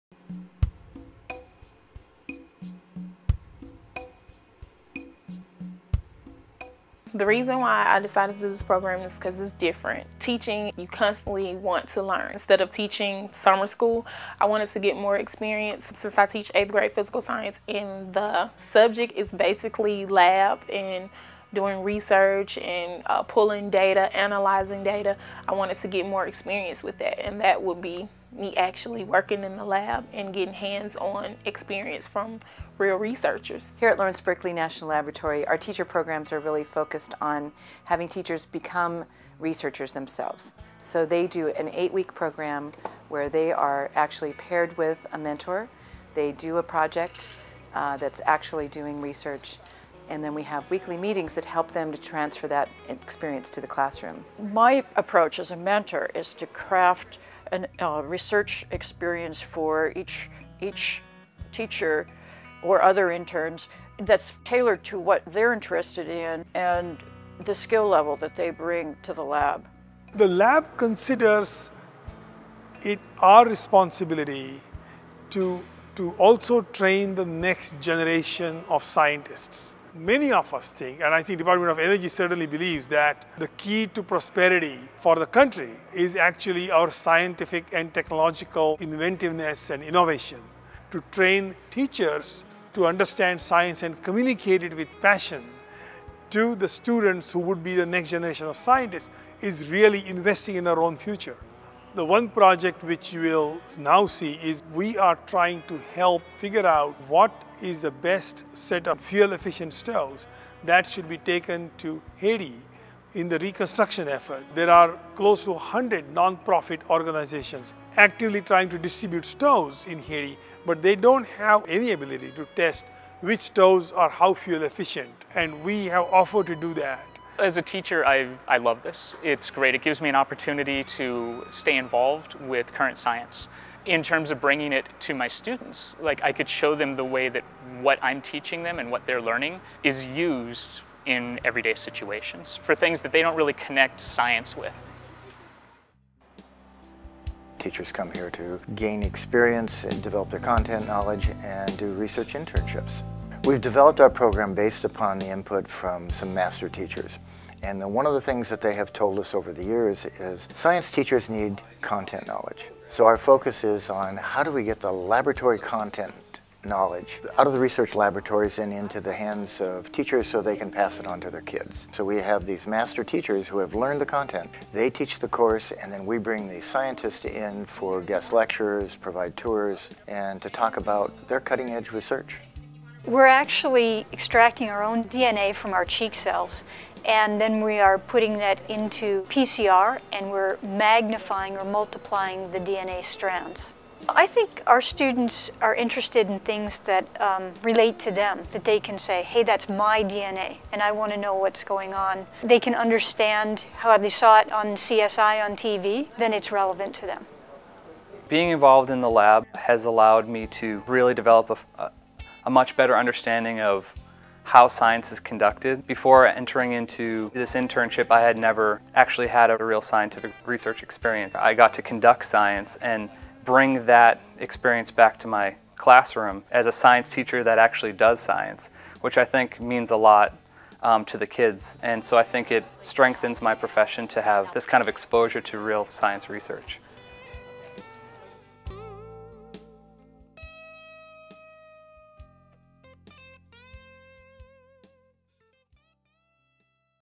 Teacher training programs in science and technology
UC offers summer programs giving high school and middle school science teachers an opportunity to work alongside researchers to advance their own knowledge of scientific advancements to take back to their classrooms. This video focuses on the Department of Energy's nationwide program to advance science in the schools.